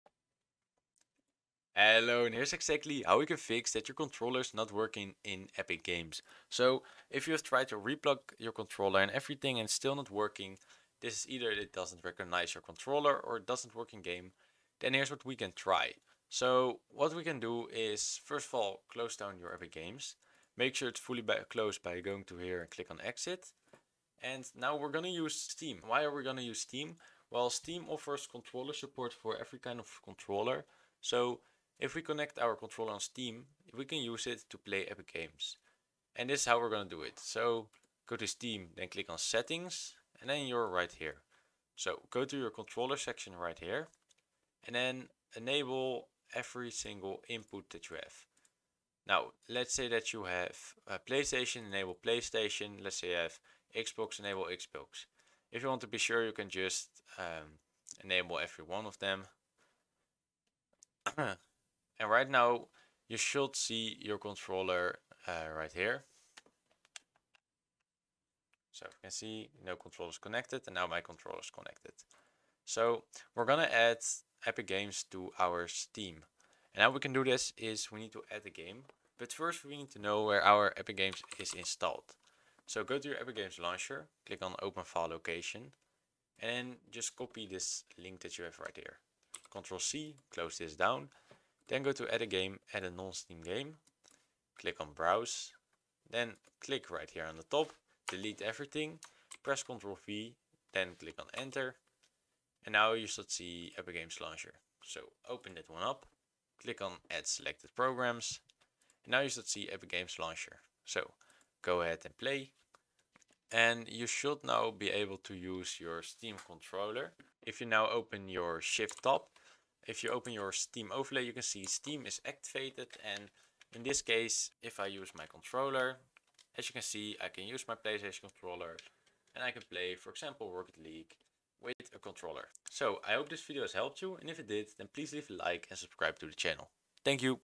Fix Controller Not Working In Epic Games - Easy Fix
Frustrated with your controller not cooperating in Epic Games? Don't worry; we've got a simple and effective solution for you! In this quick guide, we'll show you an easy fix to get your controller up and running smoothly in Epic Games, ensuring you have a hassle-free gaming experience.

🎮 What You'll Learn:

Identifying common controller issues in Epic Games.
Step-by-step instructions to resolve controller problems.
Tips to ensure your controller works seamlessly in all your favorite games.
Get back to gaming with your controller as it should be.